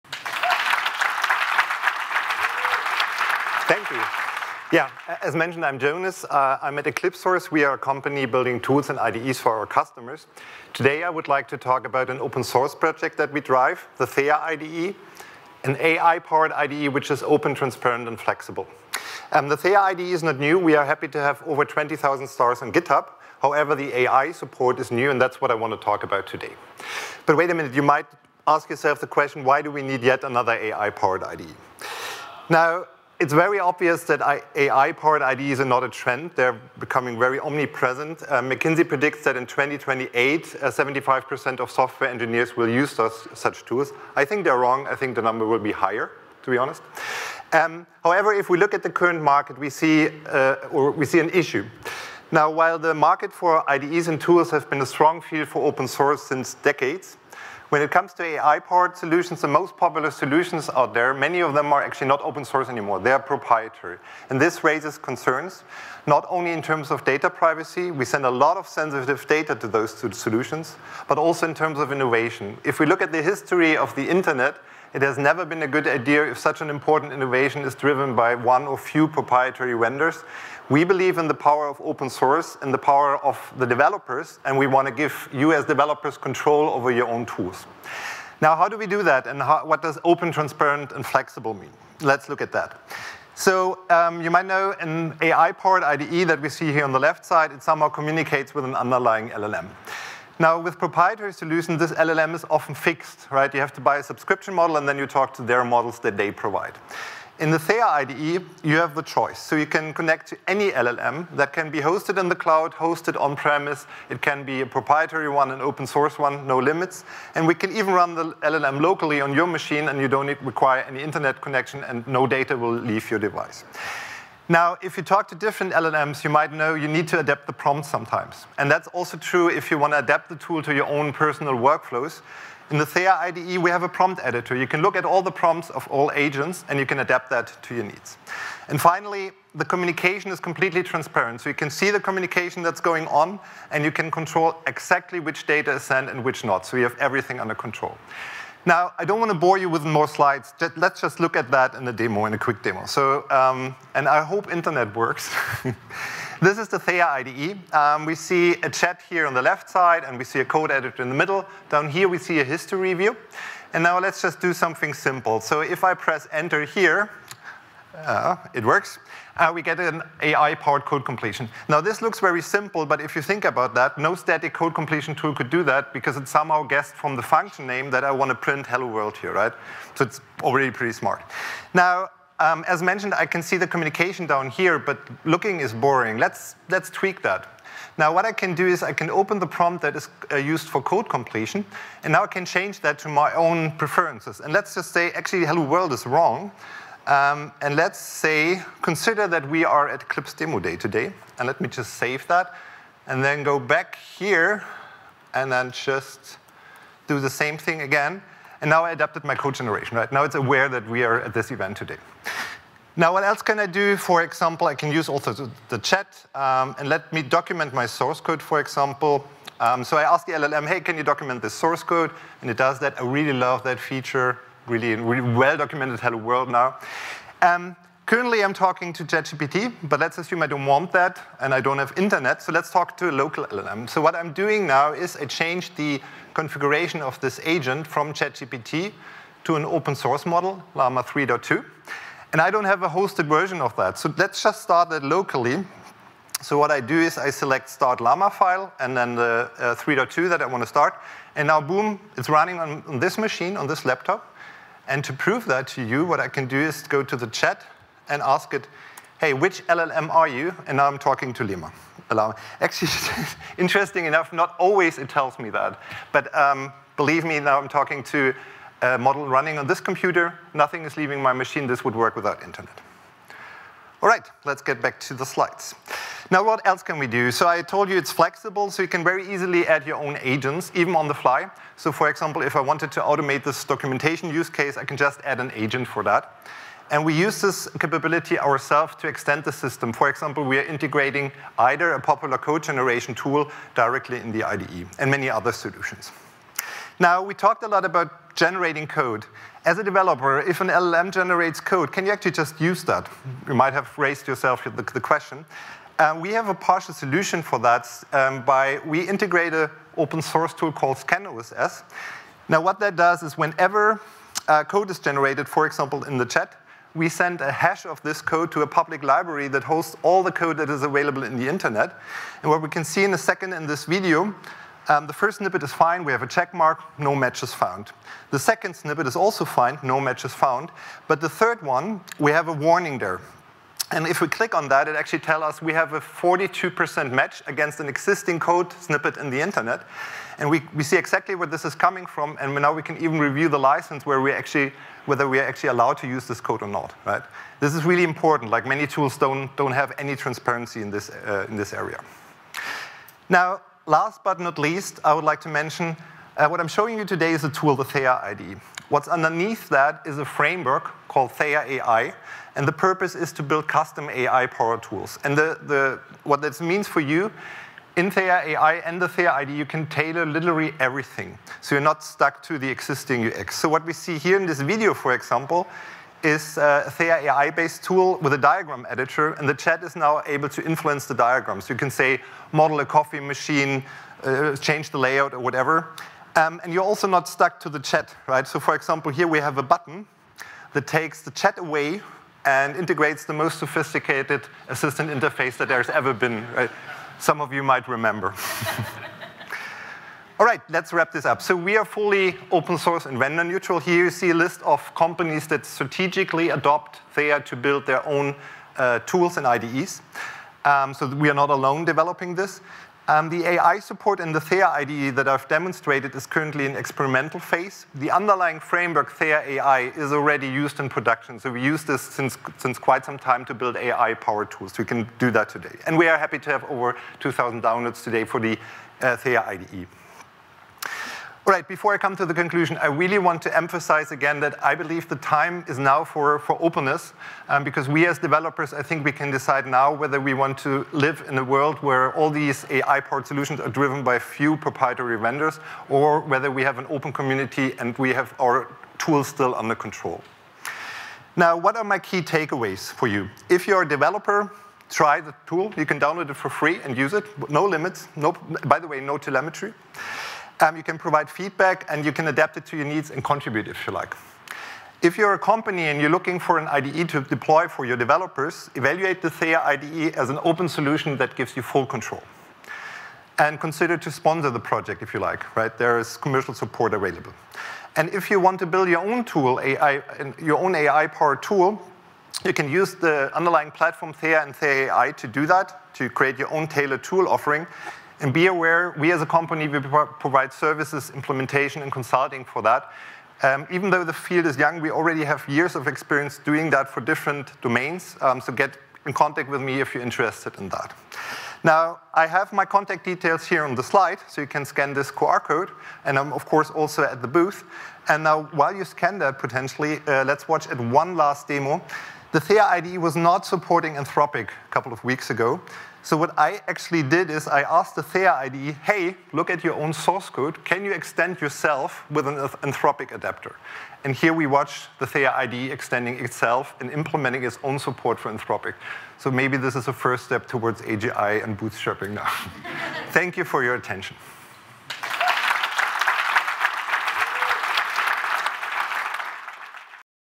An IDE That Thinks With You — Theia AI IDE
Jonas Helming digs deep into Theia AI IDE and showcases what makes its suite of AI agents trustworthy, performant, and a secret weapon for savvy devs.

Mozilla Builders helps developers create transformative open-source AI projects through collaborations, programming, and community.